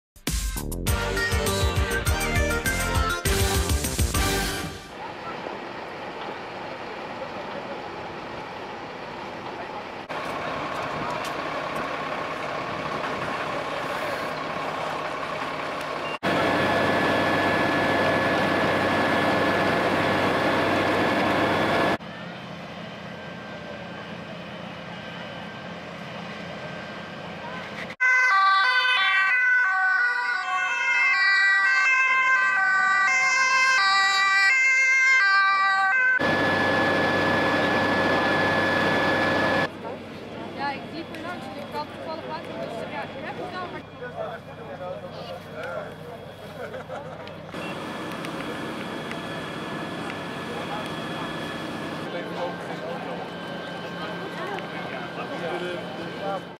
Brand in trein bij station Abcoude
Vlakbij het station van Abcoude is donderdagmiddag een kleine brand uitgebroken in een trein. Volgens de brandweer is daarbij een gewonde gevallen die naar het ziekenhuis is gebracht. Een conducteur en 23 passagiers hebben rook ingeademd en zijn ter plaatse behandeld. Daarna konden zij hun reis vervolgen. Alle overige passagiers hebben de trein veilig en ongedeerd kunnen verlaten.